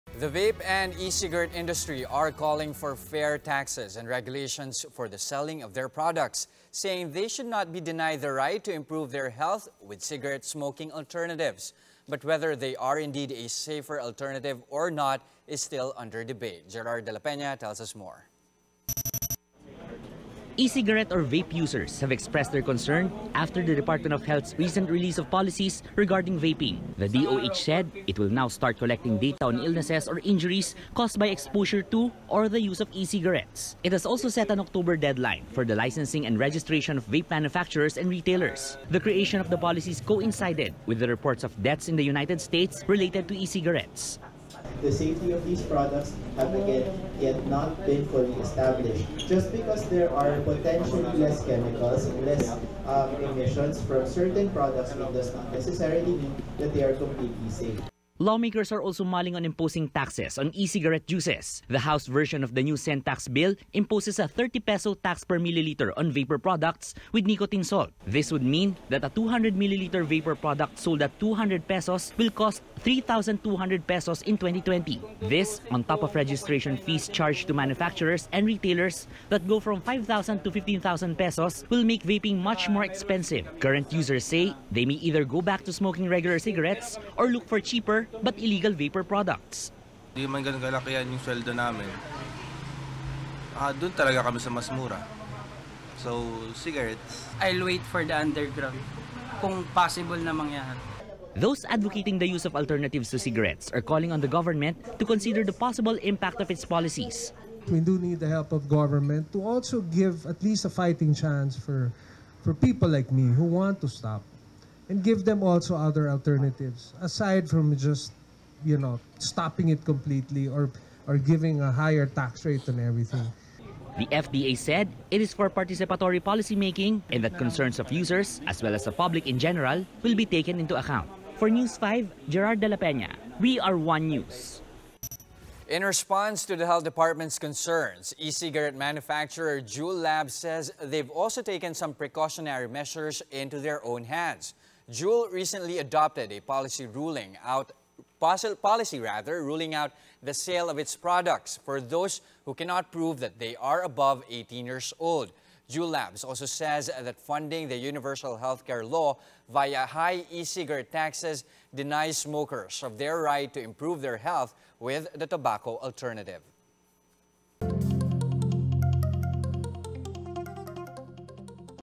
Vape, e-cig users call for 'fair' taxes
Some e-cigarette users say they might be forced to go back to smoking regular cigarettes due to the looming tax hike on vapor products.